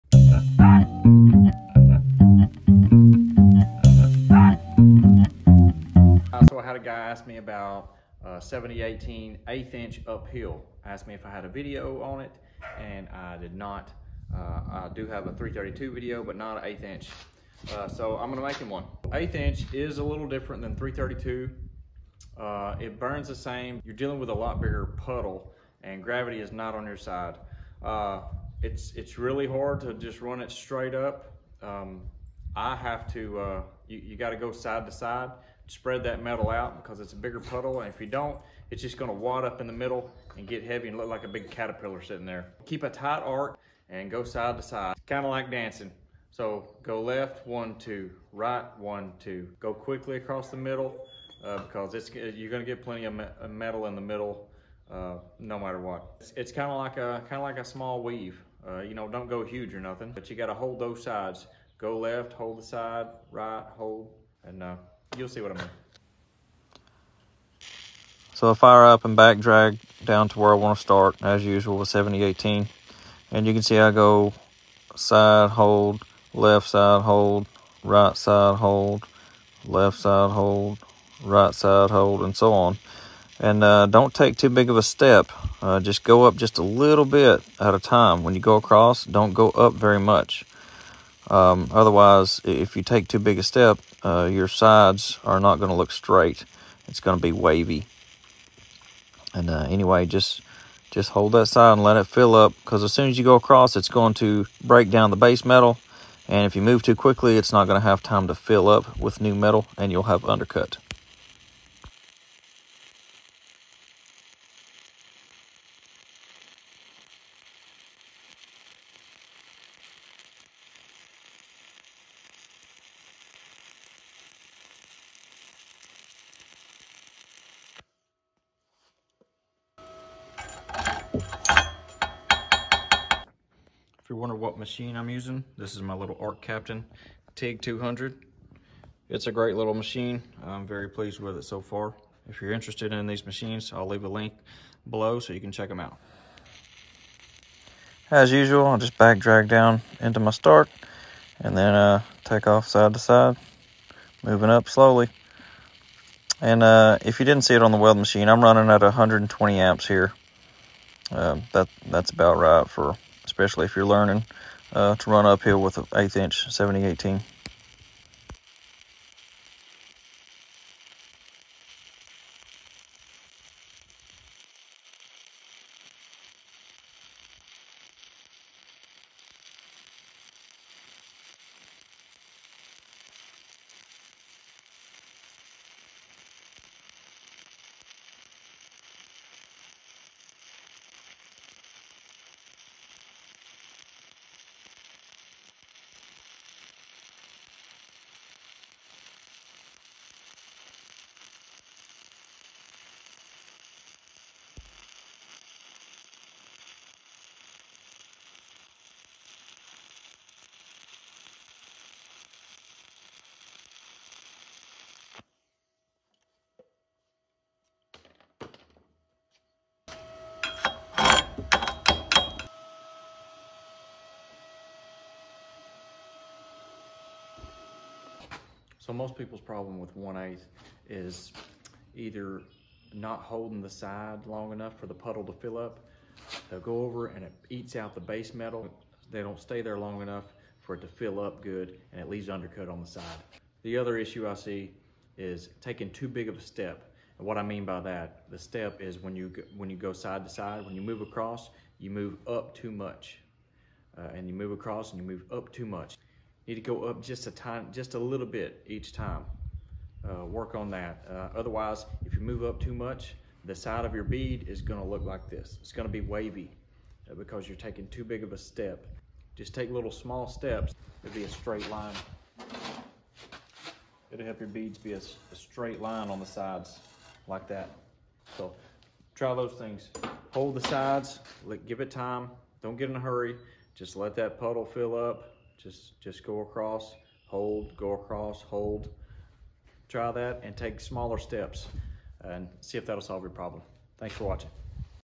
Master 1/8” uphill 7018 stick welding
This is a how to video on welding 7018 1/8” stick rod uphill. Most people struggle with undercut and knotty looking welds when going uphill.